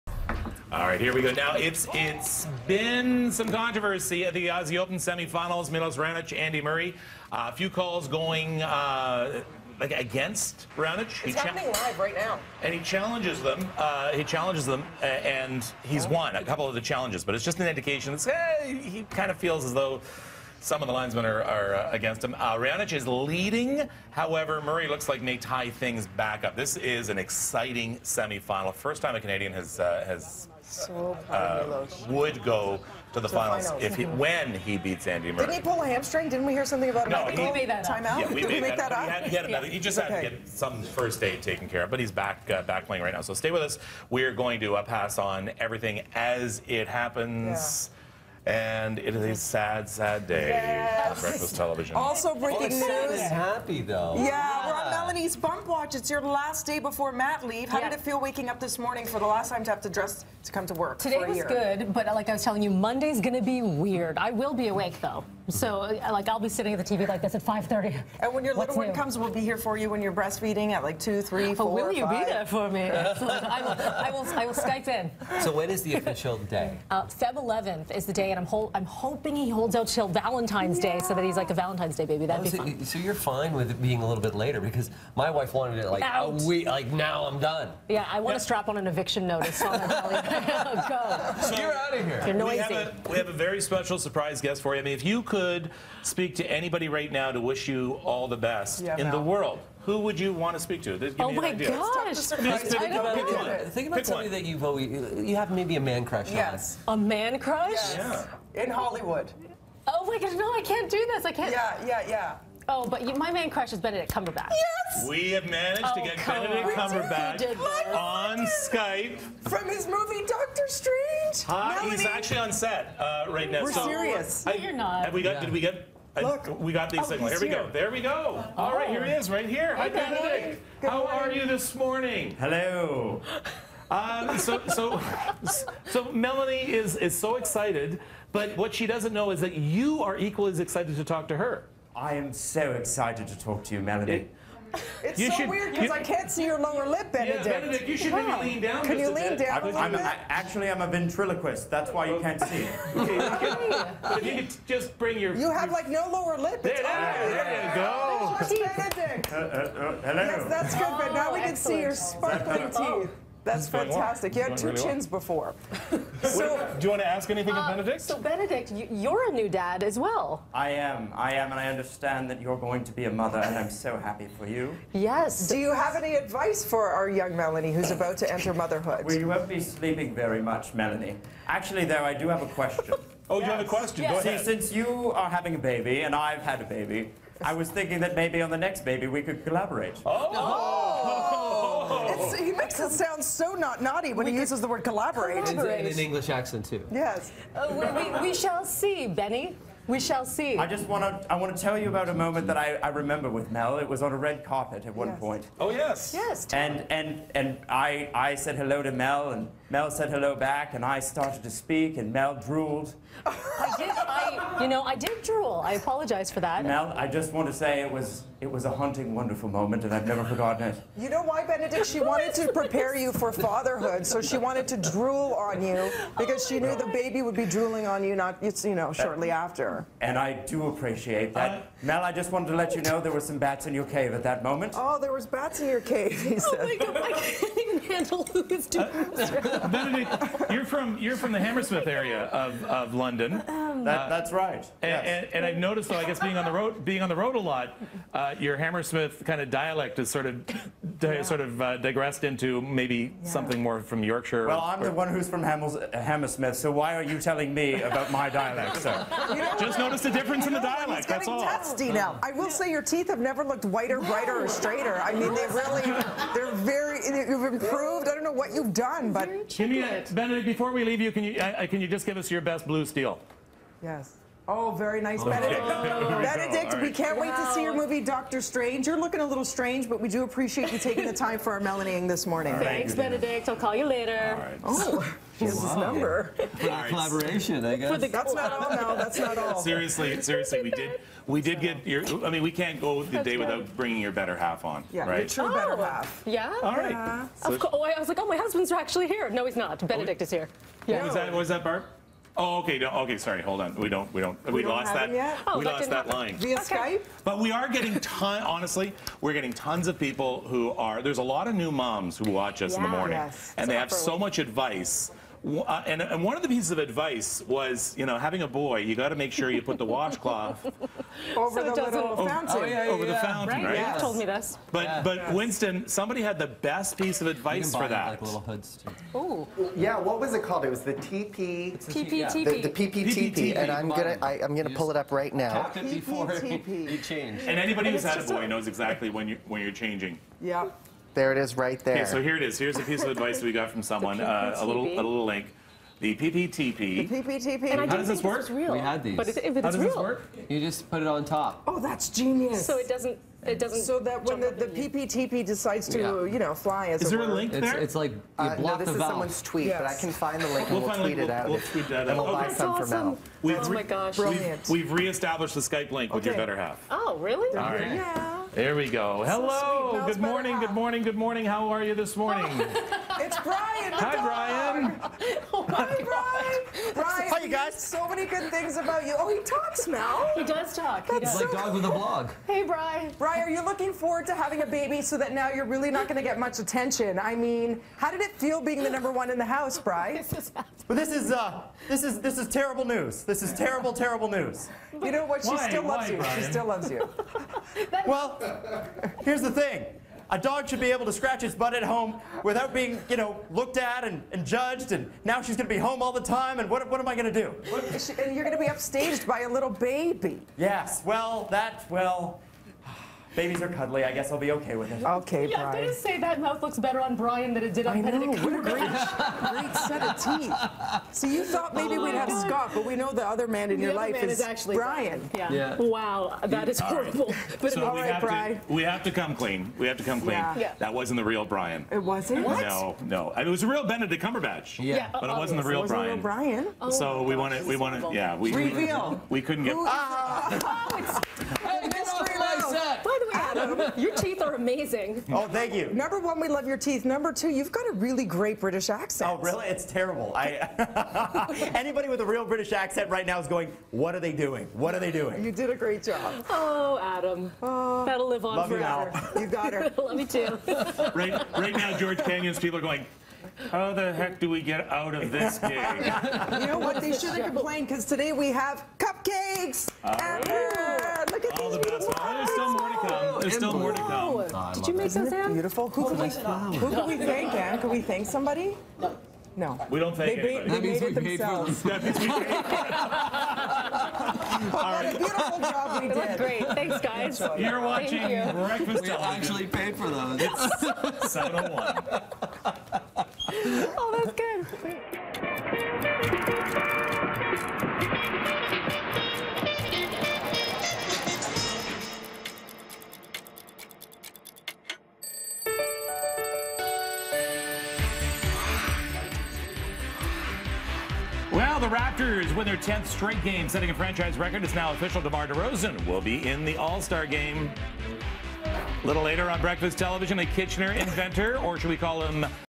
Benedict Cumberbatch and Melanie's dog Brian check in!
They join us live via Skype!